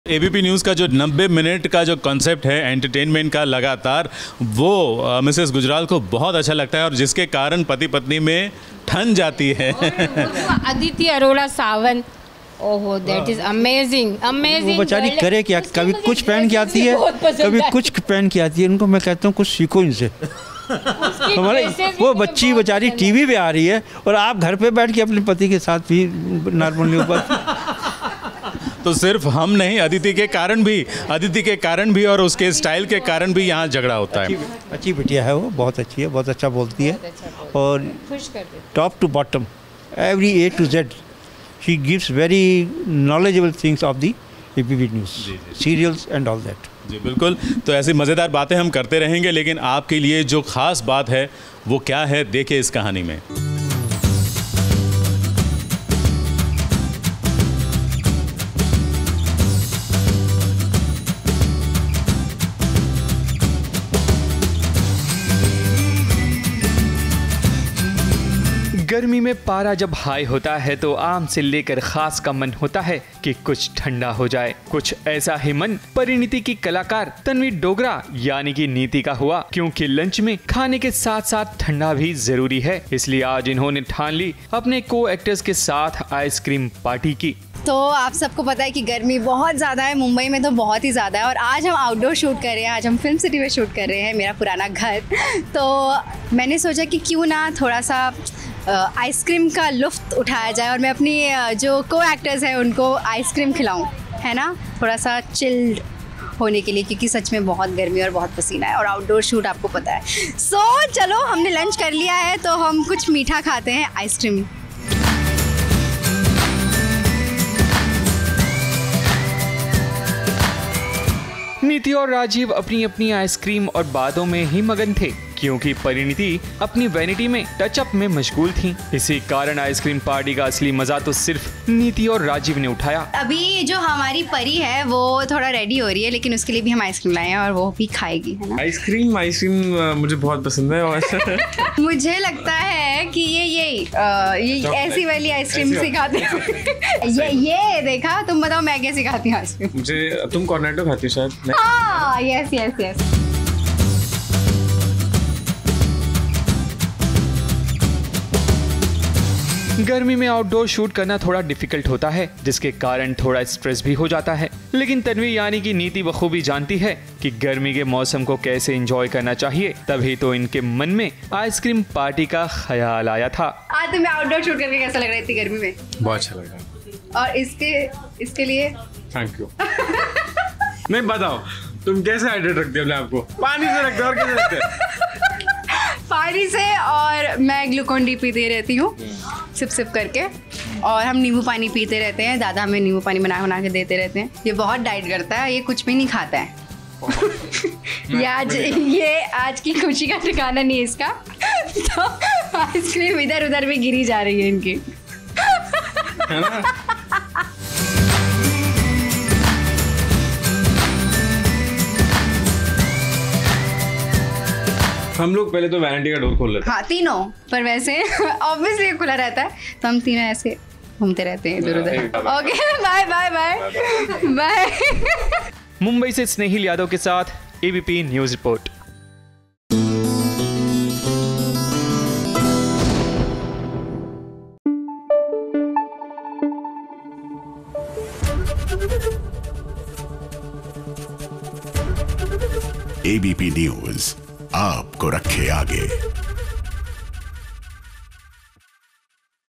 Tanvi Dogra's fun ice-cream party | SBS Originals
Tanvi Dogra is an Indian television actress. She made her acting debut with Meri Saasu Maa as Babita Sharma. Tanvi is best known for her role as Falguni Purohit in Jiji Maa. She is currently portraying Neetii Juneja in Colors TV's show Parineeti. The actress made a fun ice-cream party to refresh in the  heat.